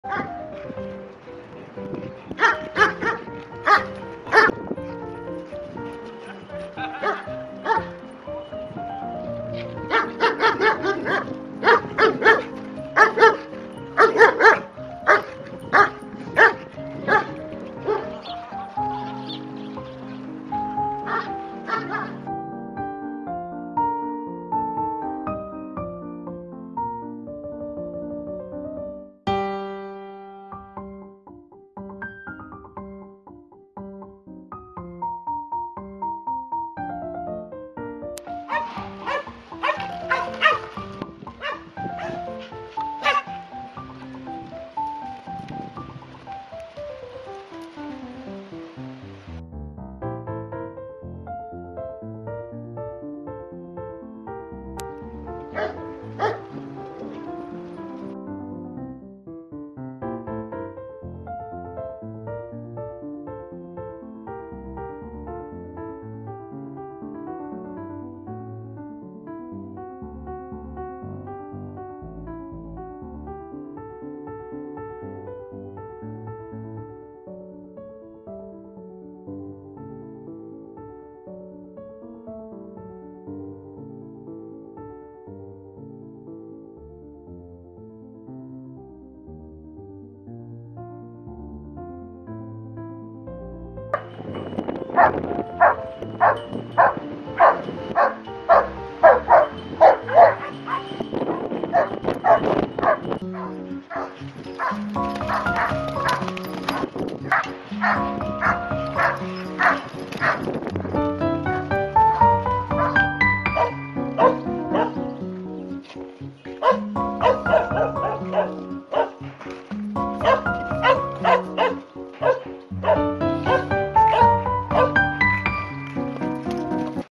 Horse Meeting 8
Horse meeting... Dog.

The funniest compilation we made of playing together horses and dogs of our friends.
Hope you enjoy watching them.